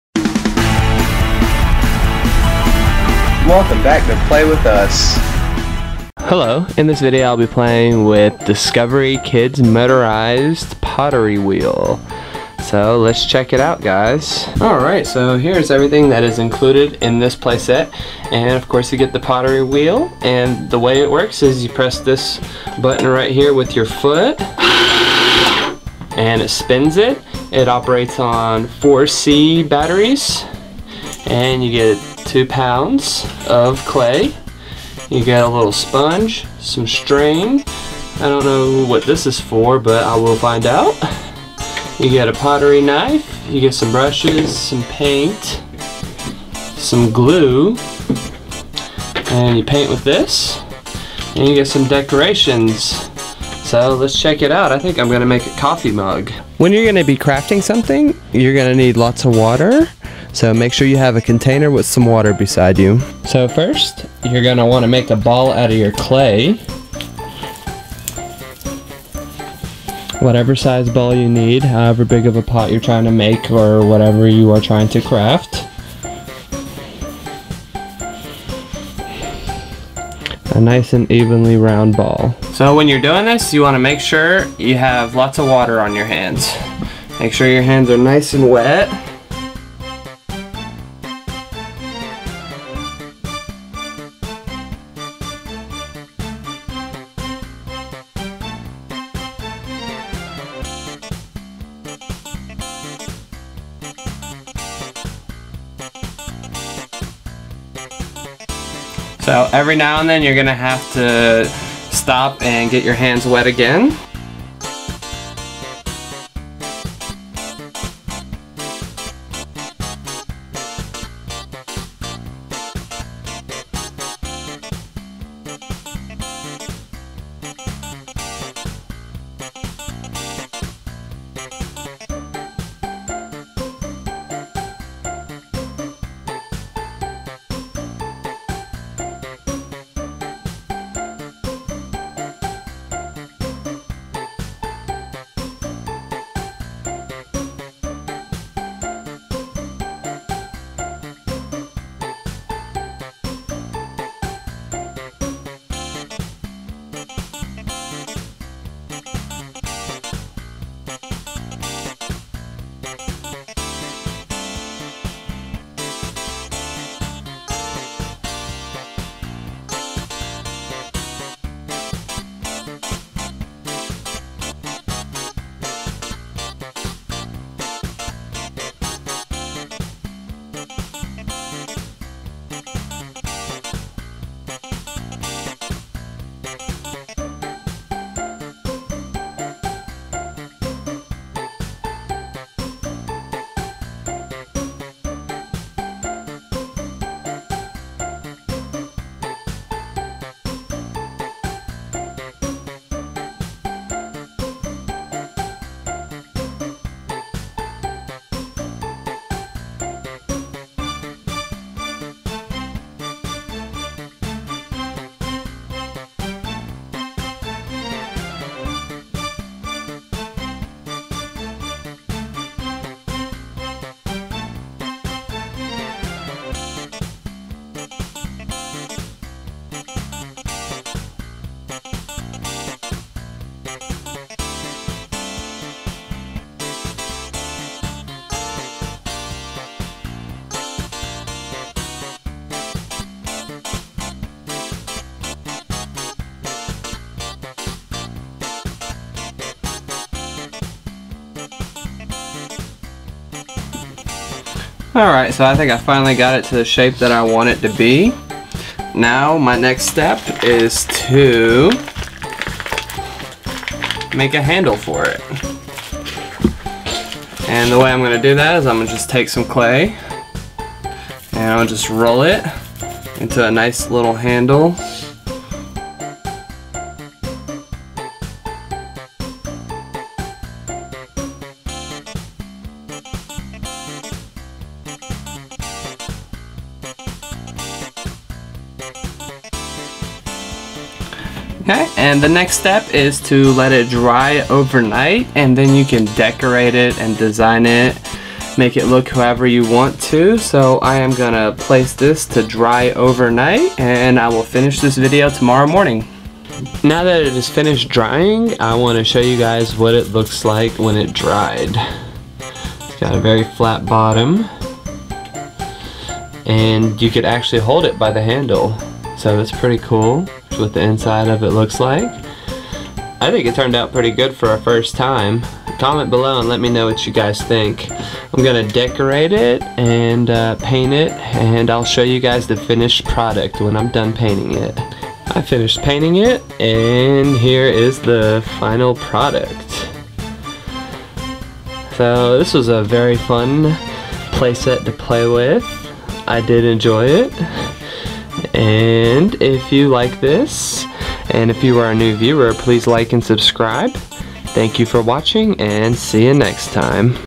Discovery Kids Motorized Pottery Wheel
Hello, In this video I'll be crafting a coffee mug using the Discovery Kids Motorized Pottery Wheel. This is my first time using a pottery wheel. Let me know in comments below how you think i did!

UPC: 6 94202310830

Also below I listed some links and titles of my other videos, Please check them out!